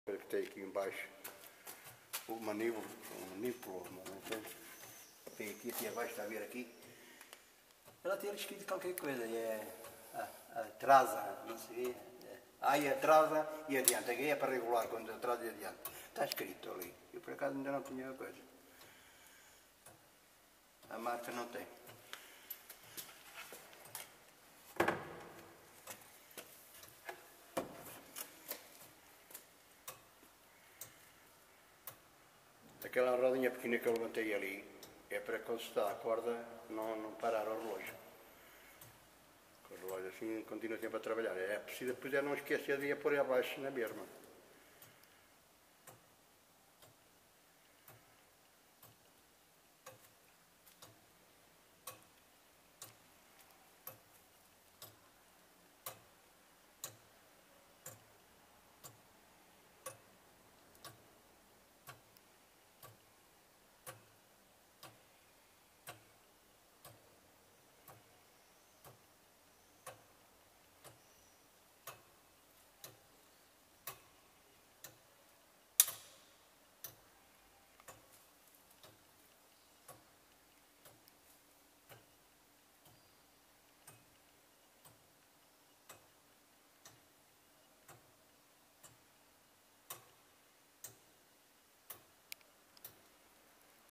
RELÓGIO MANUAL DE PESOS DA CAPELA DE SANTO ANTÓNIO EM OVAR
Mário Duarte Resende funcionário e ministro da Capela de Santo António de Ovar e responsável por dar corda ao relógio manual de pesos.